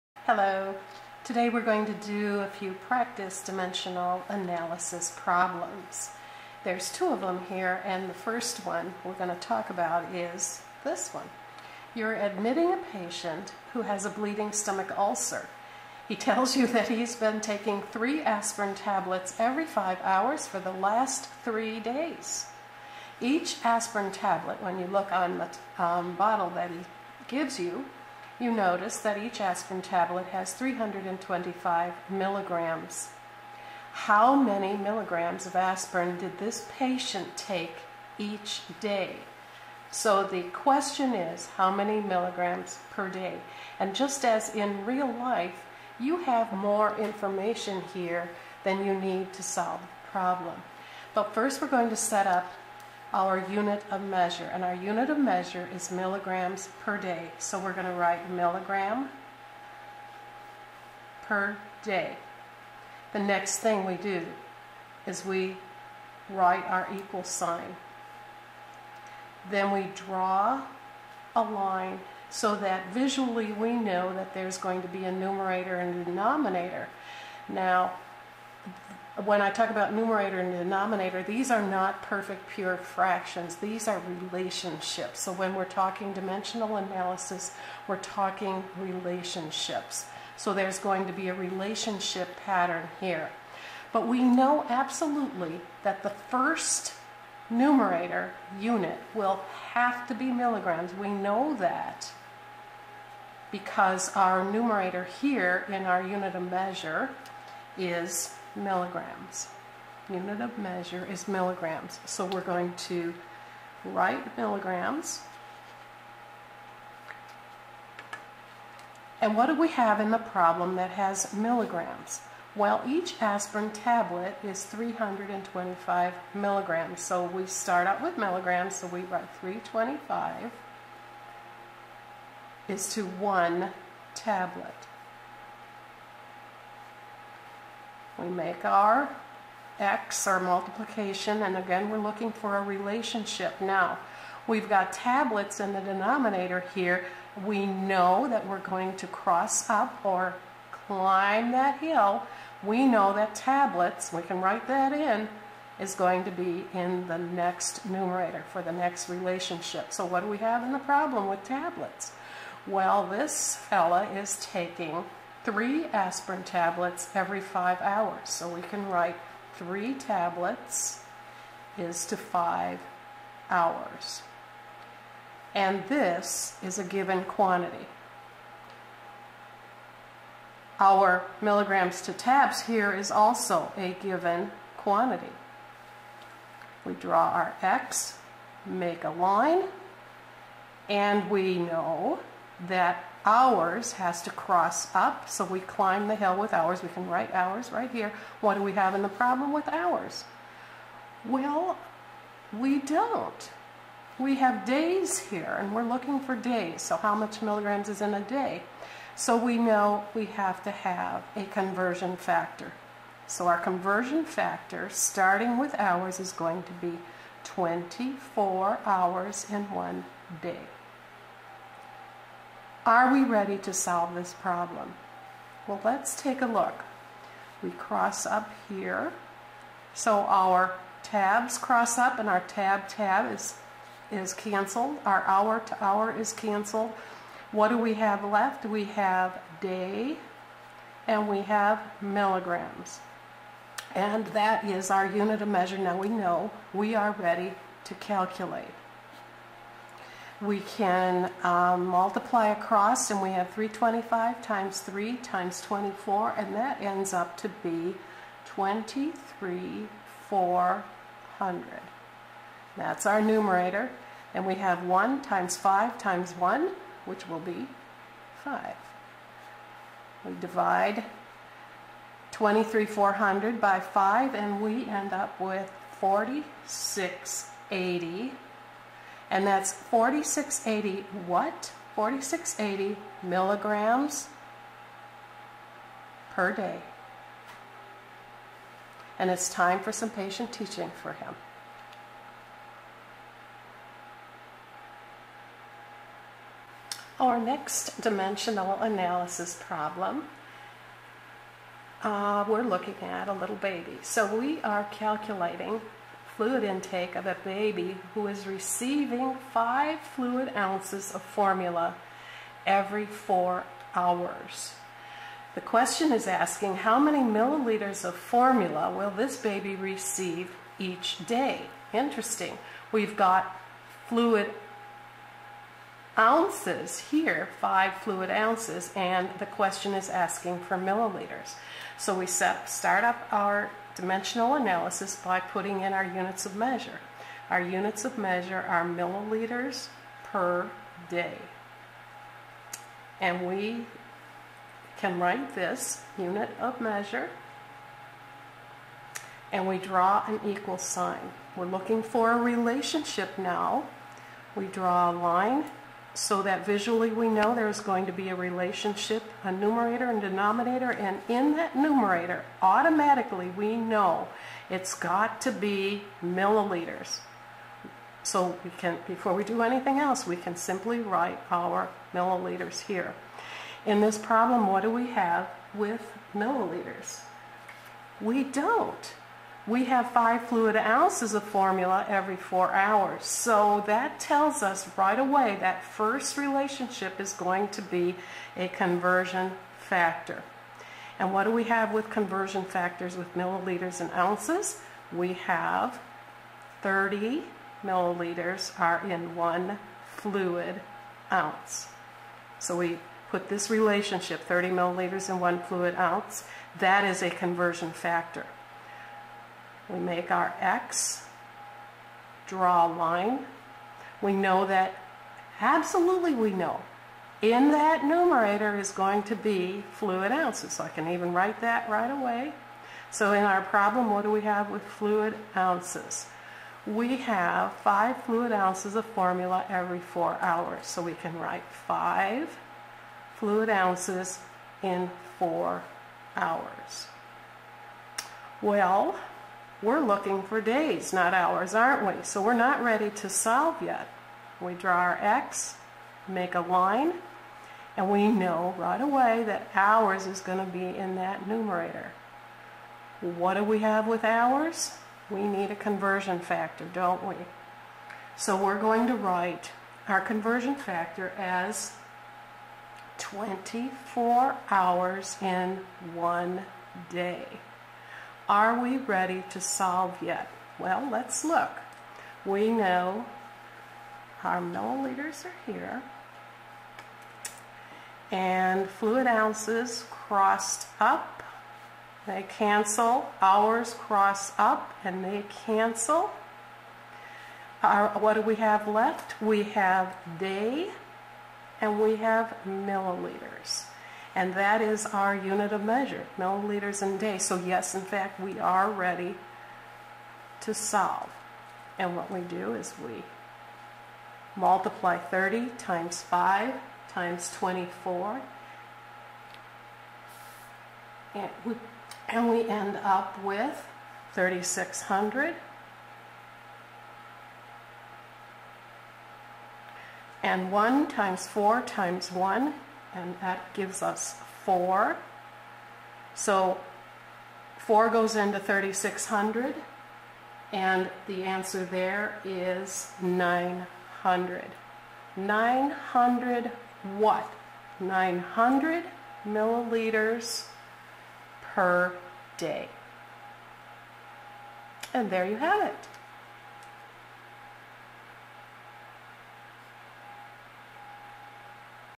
Dimensional Analysis - Basic Calculations
Two problems are identified. Dimensional Analysis techniques are used to solve two basic healthcare problems.